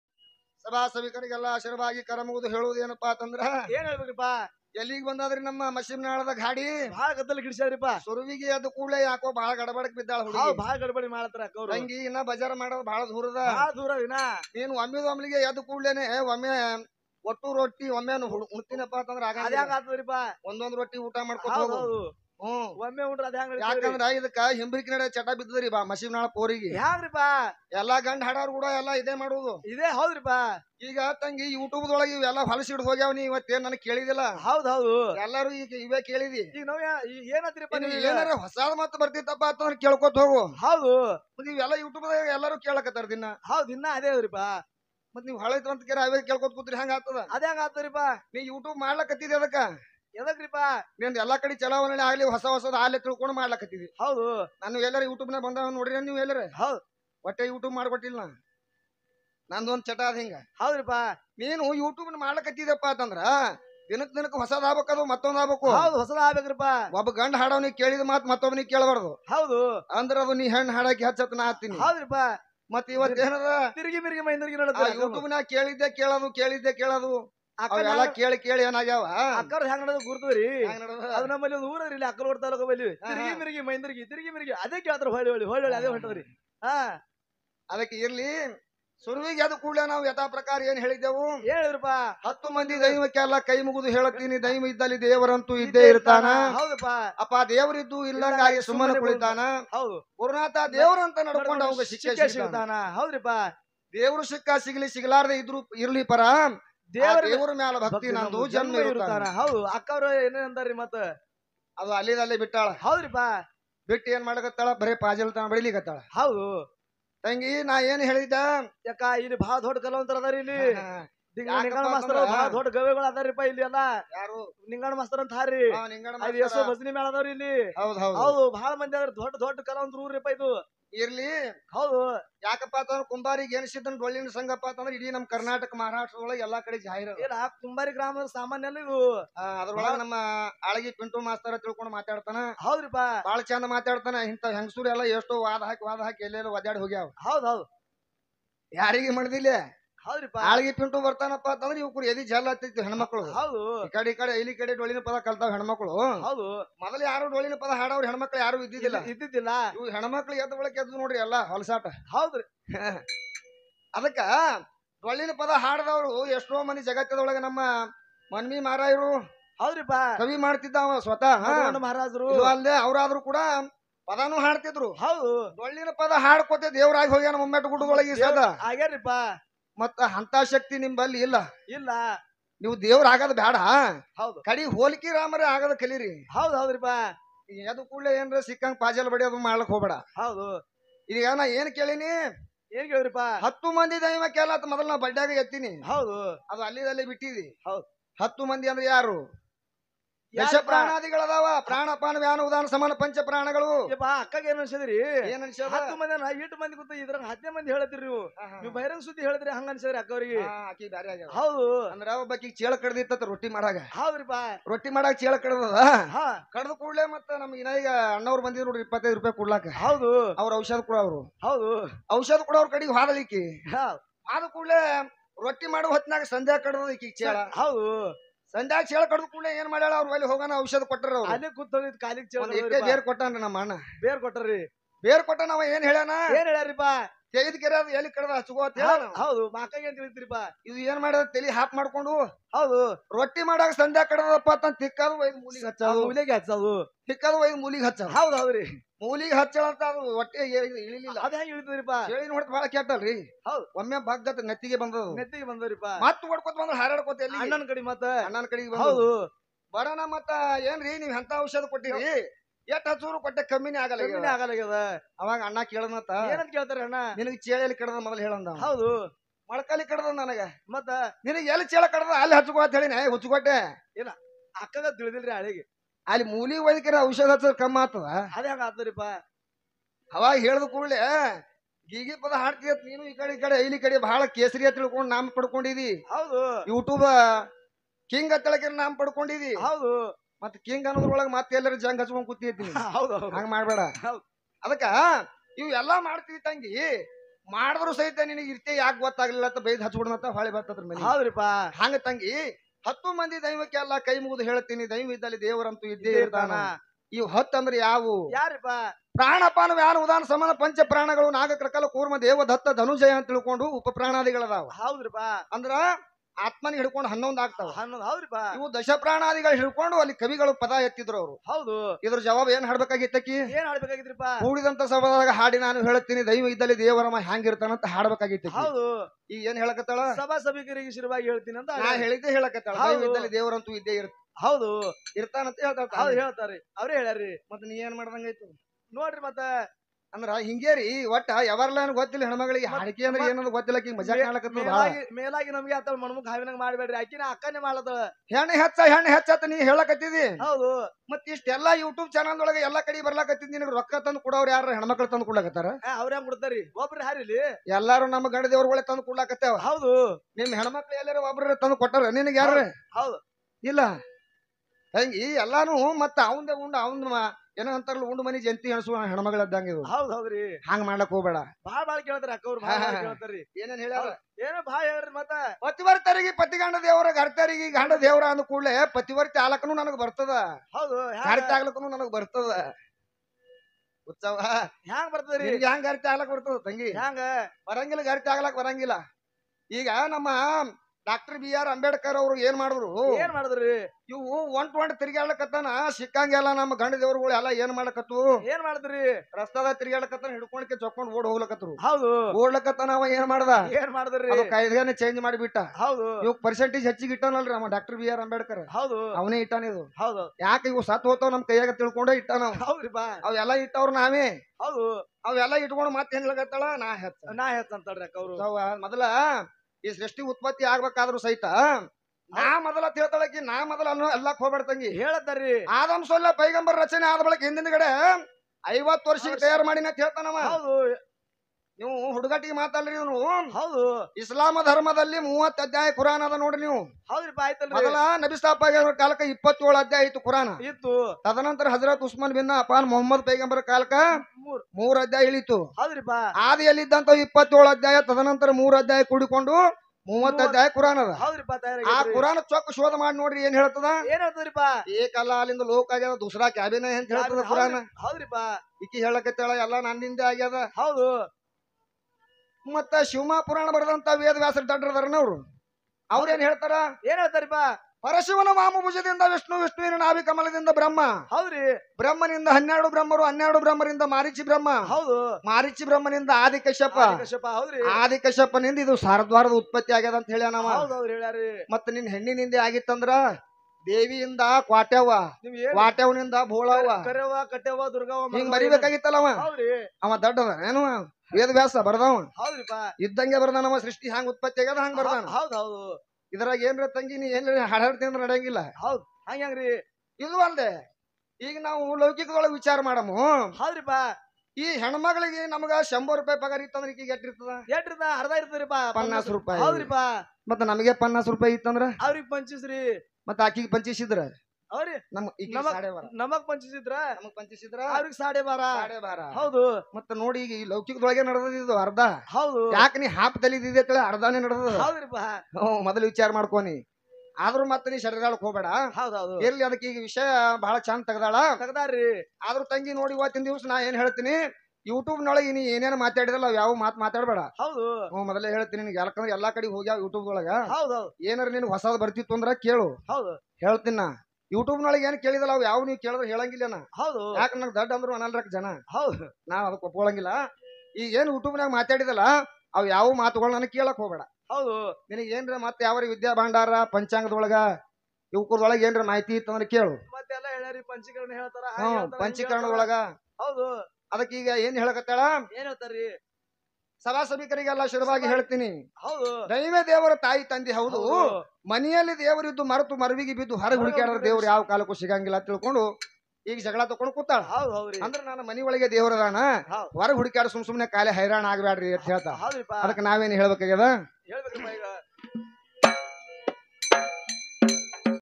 ಬಸವರಾಜ ಮದರಿ ಗೀಗೀ ಪದ 13 | basavaraja madari Gigi pad | Gigi padagalu ‎‎@RaviAudio355
Song: GIGI PADA
Singer : ಬಸವರಾಜ ಮದರಿ
Lyrics:

" ಗೀಗಿ ಪದಗಳು"

"ಕುಂಬಾರಿ"ಗ್ರಾಮದಲ್ಲಿ ನಡೆದ ಕಾರ್ಯಕ್ರಮ